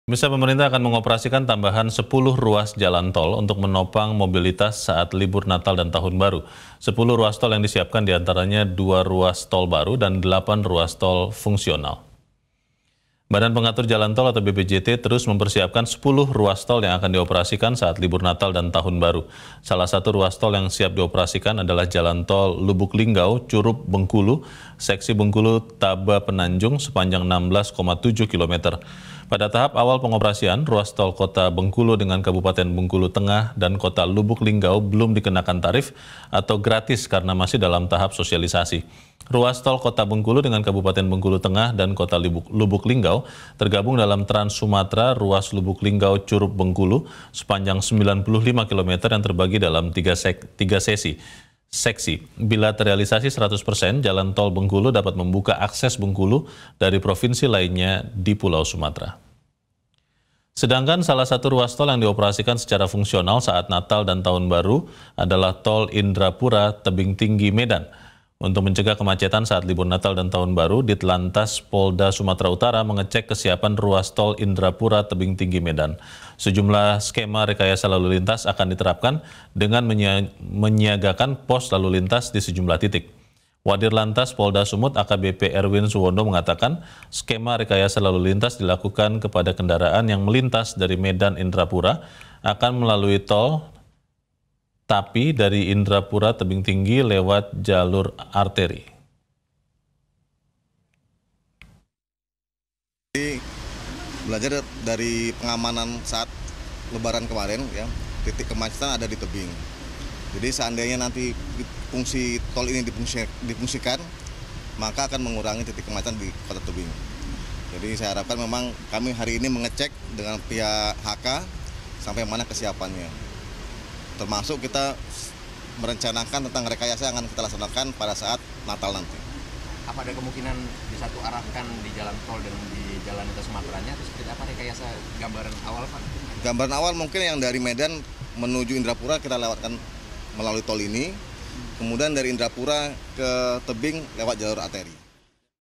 Jelang Natal dan Tahun Baru 2023, Pemerintah Akan Tambah 10 Ruas Jalan Tol di Bengkulu
MetroTV, Jelang libur Natal 2022 dan Tahun Baru 2023, pemerintah akan mengoperasikan tambahan 10 ruas jalan tol diantaranya dua ruas tol baru dan delapan ruas tol fungsional untuk menopang mobilitas saat libur Nataru.

Badan Pengatur Jalan Tol (BPJT) terus mempersiapkan 10 ruas tol yang akan dioperasikan saat libur Nataru. Salah satu ruas tol yang siap dioperasikan adalah jalan Tol Lubuk Linggau-Curup-Bengkulu seksi Bengkulu-Taba Penanjung sepanjang 16,7 kilometer.

Pada tahap awal pengoperasian, ruas tol Kota Bengkulu dengan Kabupaten Bengkulu Tengah dan Kota Lubuk Linggau belum dikenakan tarif atau gratis karena masih dalam tahap sosialisasi.

Ruas tol Kota Bengkulu dengan Kabupaten Bengkulu Tengah dan Kota Lubuk Linggau tergabung dalam Trans Sumatra ruas Lubuk Linggau-Curup-Bengkulu sepanjang 95 kilometer yang terbagi dalam tiga seksi. Bila terealisasi 100%, jalan Tol Bengkulu dapat membuka akses Bengkulu dari provinsi lainnya di Sumatra.